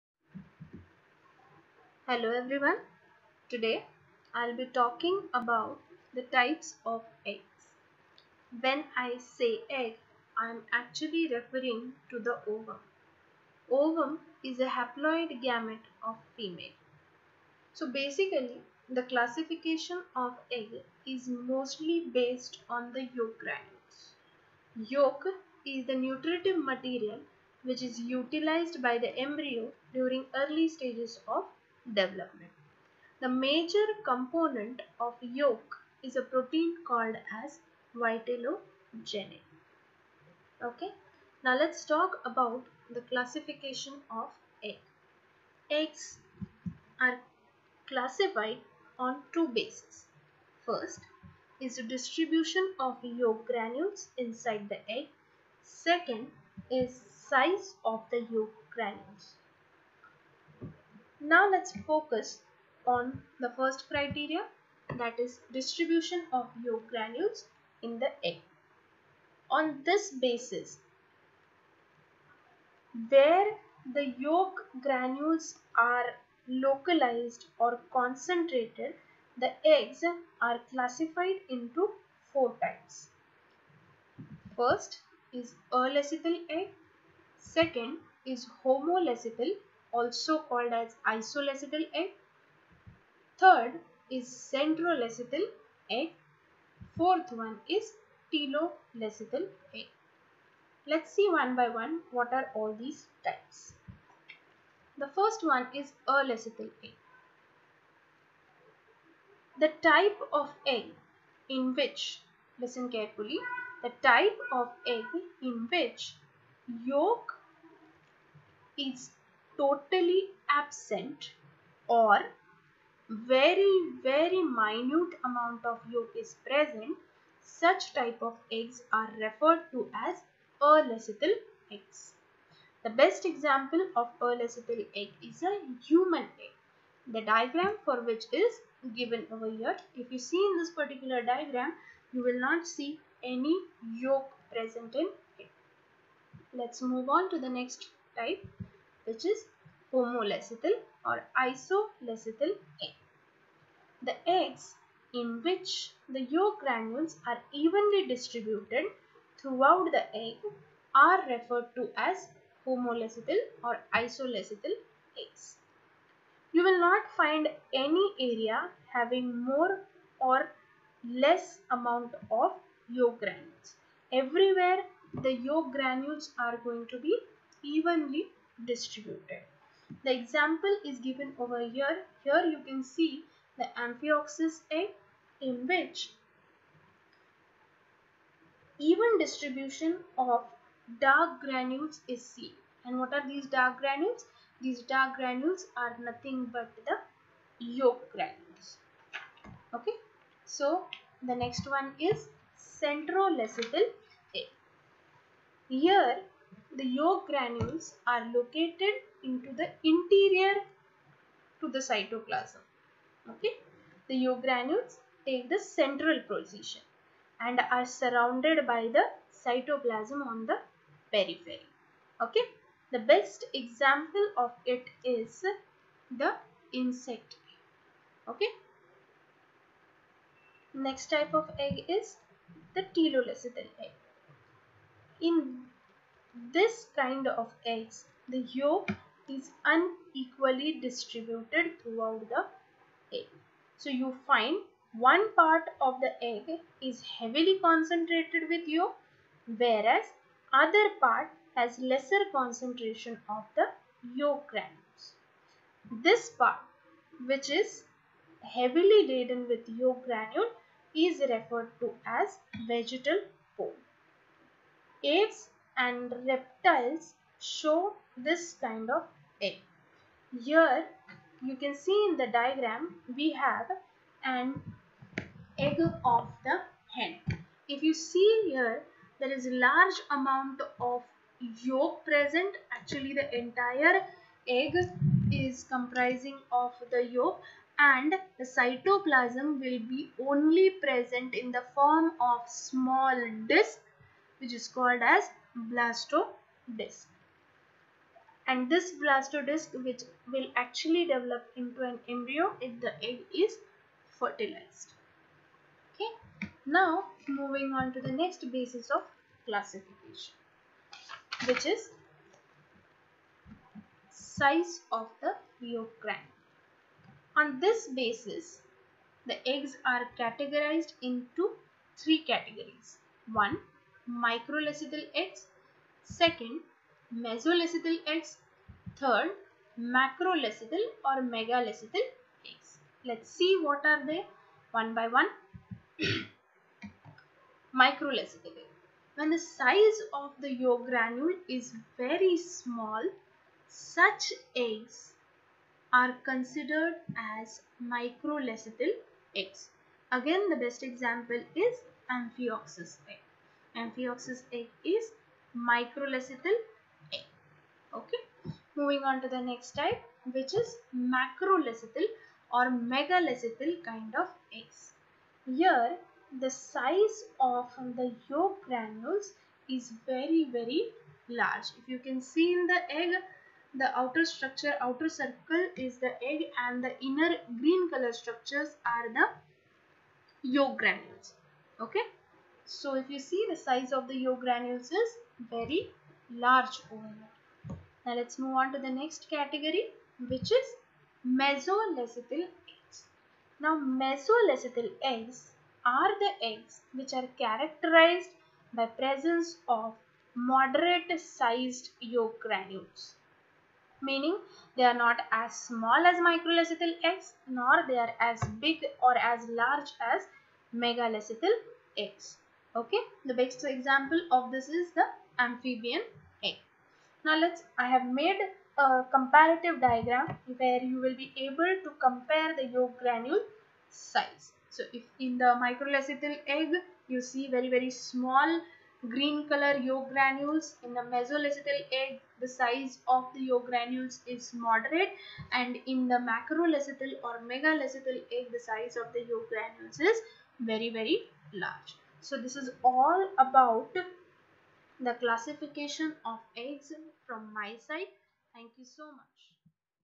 Types of eggs
Classification of eggs based on distribution and size of yolk granules.